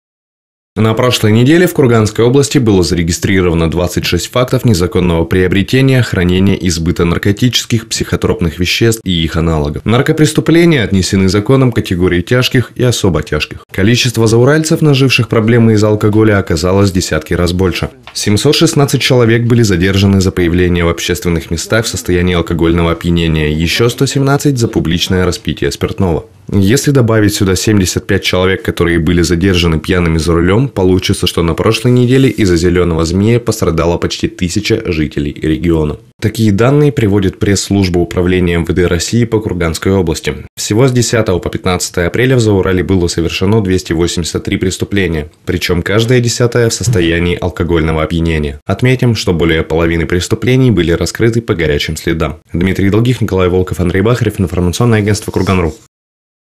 «Зеленый змий» на прошлой неделе опутал почти тысячу курганцев
Жители Курганской области предпочитают наркотикам алкоголь